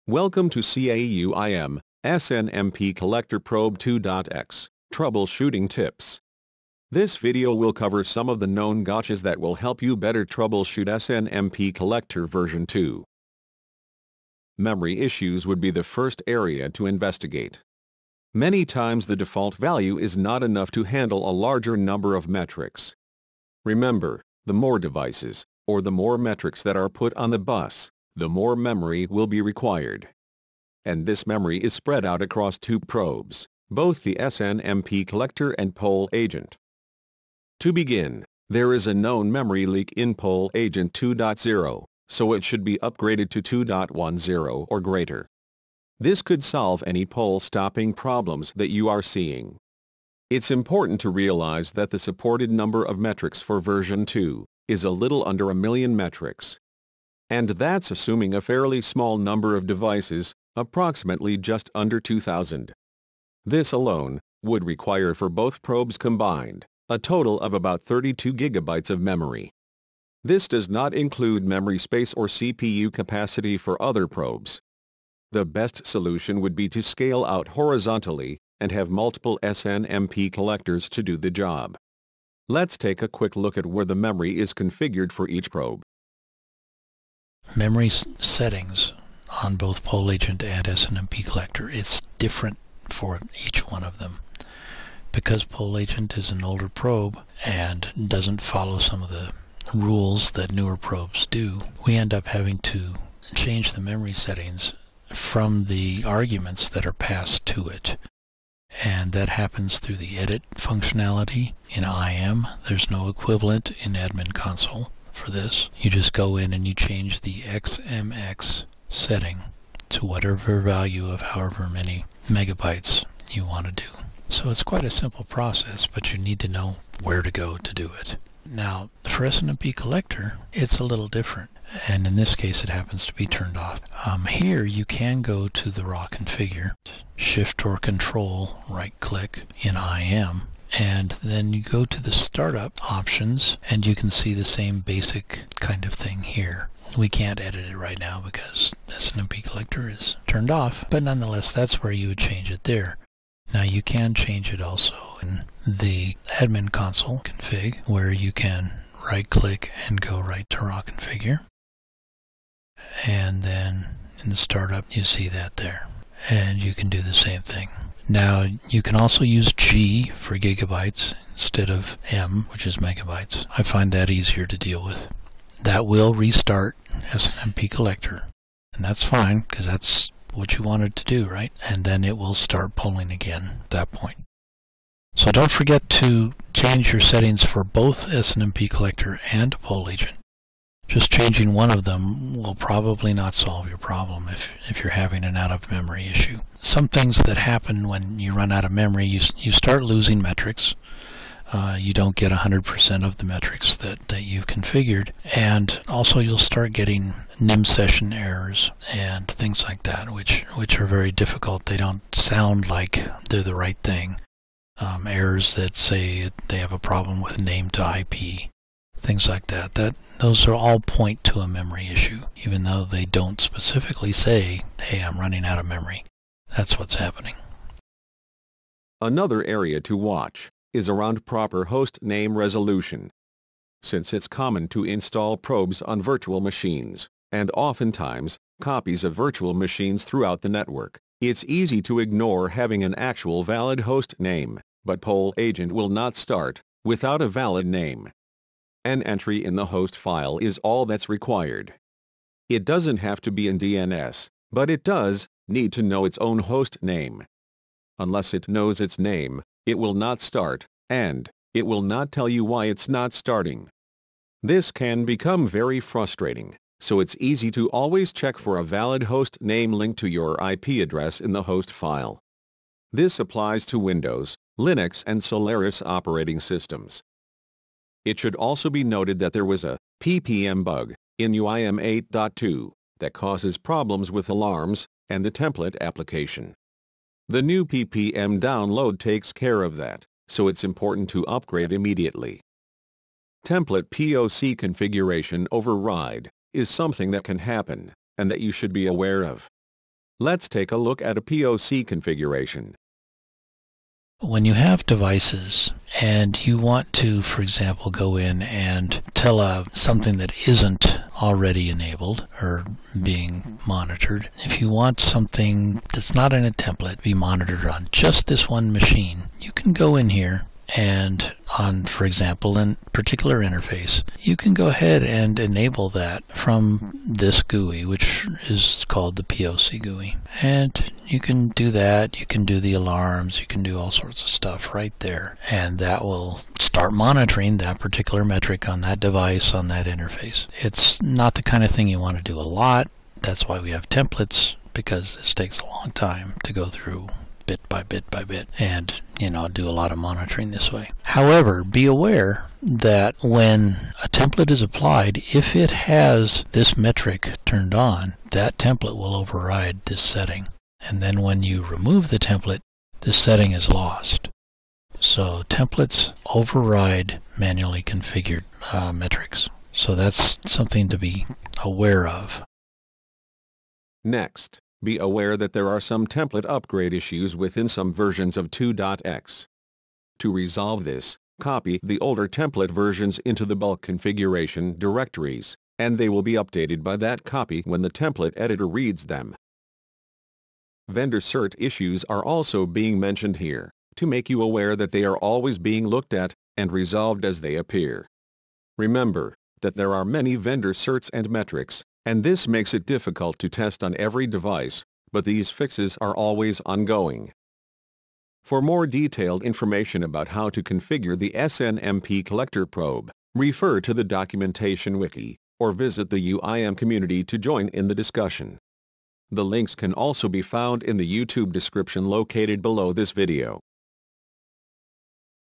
snmpcollector Probe Troubleshooting Tips
This video will cover some of the known gotchas that will help you better troubleshoot snmpcollector 2.x. For more detailed information, refer to Probe Documentation or visit the CA Communities to join in the discussion!